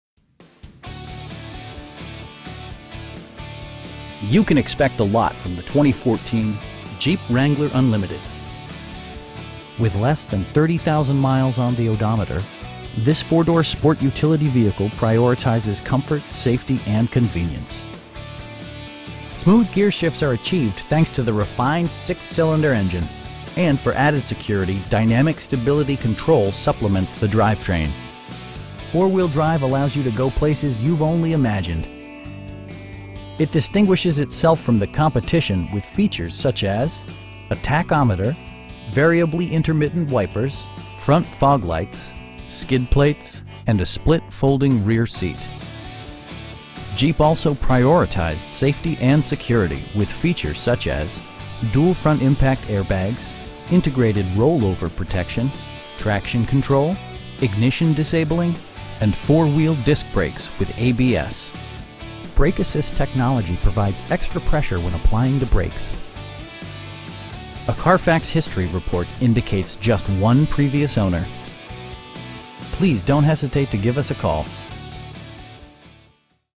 2014 Jeep Wrangler Unlimited Sport
You can expect a lot from the 2014 Jeep Wrangler Unlimited. With less than 30,000 miles on the odometer, this 4 door sport utility vehicle prioritizes comfort, safety and convenience. Smooth gearshifts are achieved thanks to the refined 6 cylinder engine, and for added security, dynamic Stability Control supplements the drivetrain. Four wheel drive allows you to go places you've only imagined. It distinguishes itself from the competition with features such as: a tachometer, variably intermittent wipers, front fog lights, tilt steering wheel, skid plates, cruise control, and a split folding rear seat. Jeep also prioritized safety and security with features such as: dual front impact airbags, integrated roll-over protection, traction control, ignition disabling, and 4 wheel disc brakes with ABS. Brake assist technology provides extra pressure when applying the brakes. A Carfax history report indicates just one previous owner. We pride ourselves in the quality that we offer on all of our vehicles. Please don't hesitate to give us a call.